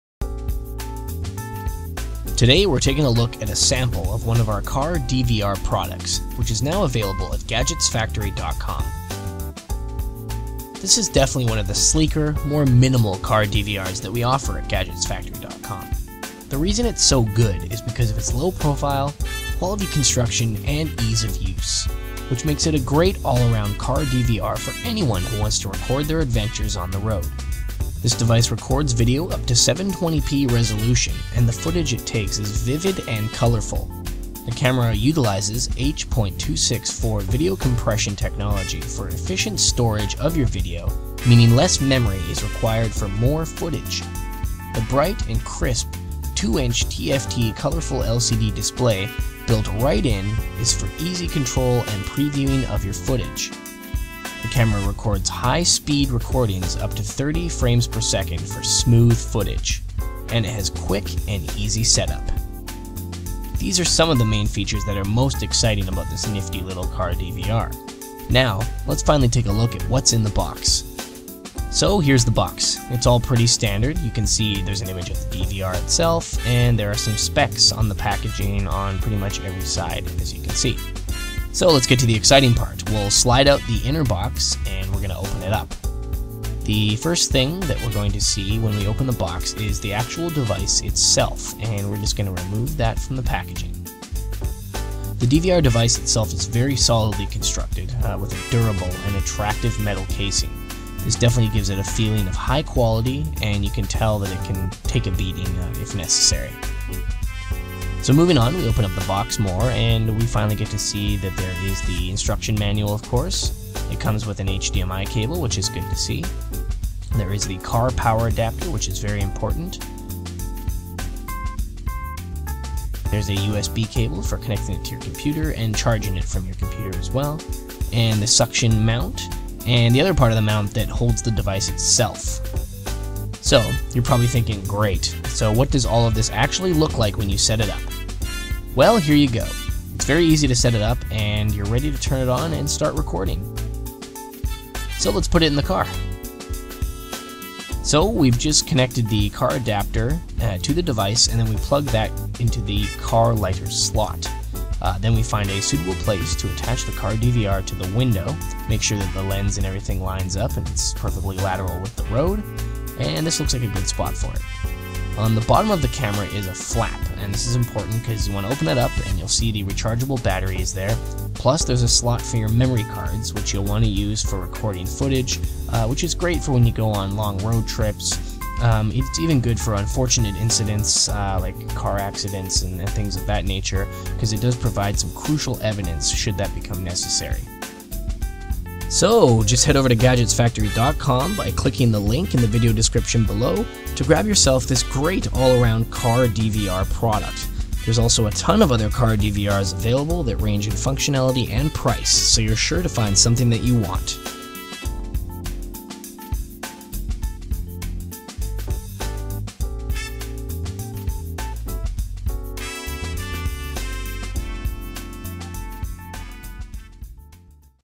Black Box Camera for The Car - Easy To Use Car DVR
Click the link above to grab one today!

Record everything and never miss those memorable moments on the road.
A Car DVR is a car camera that enables you to safely record video footage while you travel on the road.

If you're heading off on a fun road trip, grab a Car DVR and record it. Save the footage, edit it, and upload it to YouTube for everyone to enjoy. What a fantastic way to tell a story of your trip!

Car DVRs also function as a black box camera for your car, recording crucial evidence during unfortunate accidents -- Just like a black box on a plane.

These car cameras are mounted onto your window or dashboard, where they can best view all the action! The image resolution varies from model to model, but they are all perfect for recording live footage in vivid quality. With our selection of Car DVRs you're ready for every situation!

LIMITED TIME ONLY. Free Express Shipping will be offered on many of our Car DVRs!